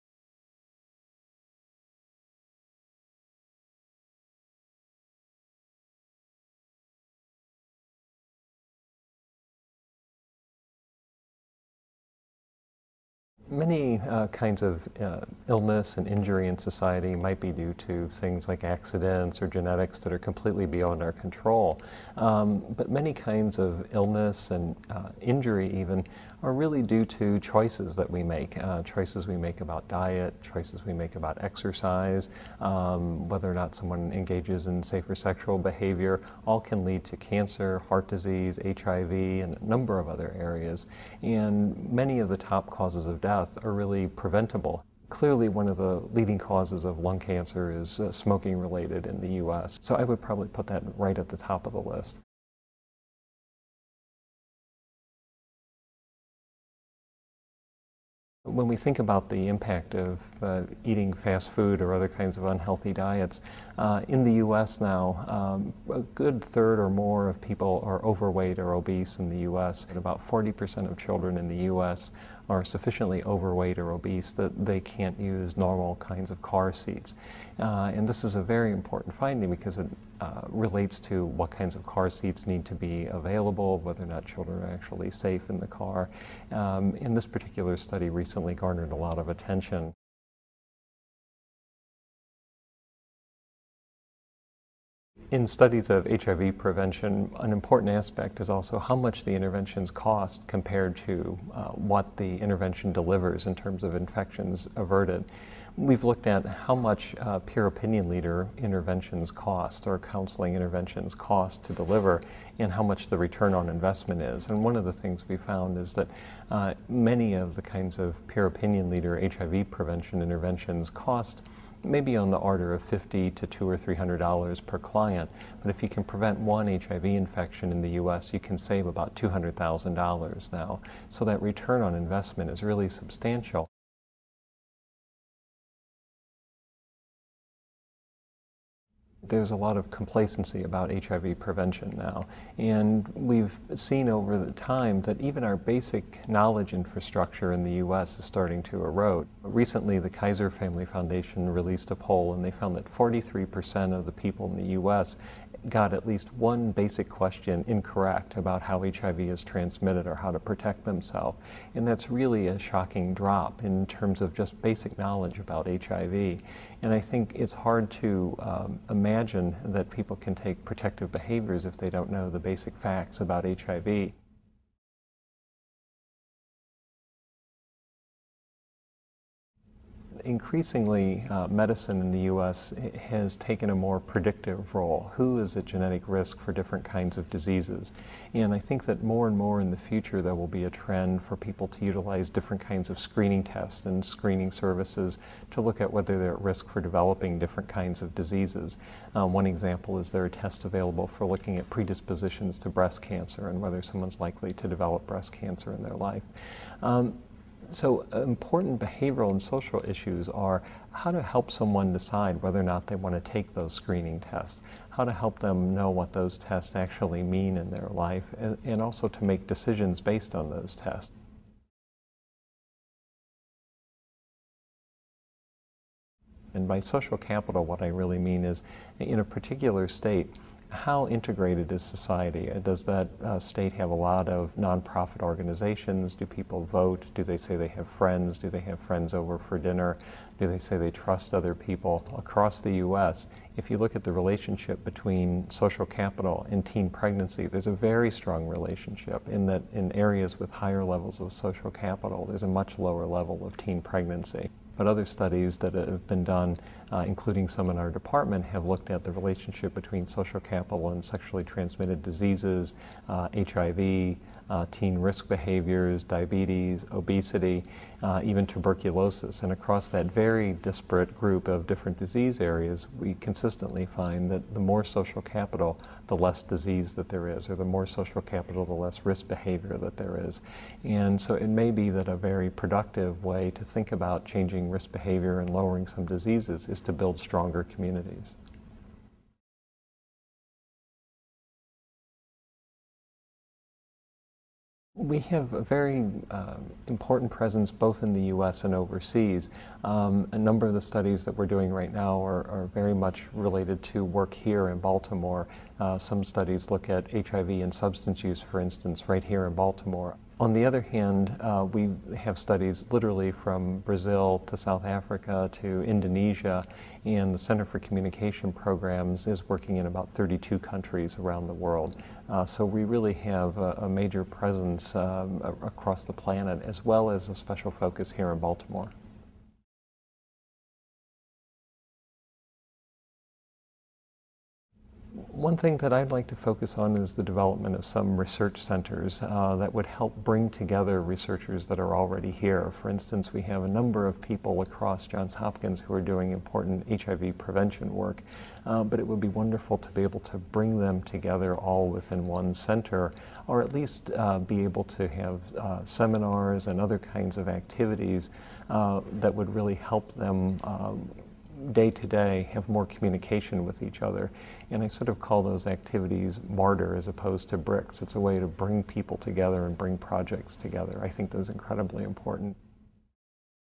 Public Health Issues from David Holtgrave of Johns Hopkins
David Holtgrave, PhD, chair of the Department of Health, Behavior and Society at the Bloomberg School of Public Health, talks about public health issues faculty are studying: how choices and behavior affect public health, child obesity, HIV prevention, genetic counseling, and social capital. He also speaks to the importance of local and global programs and more research collaboration.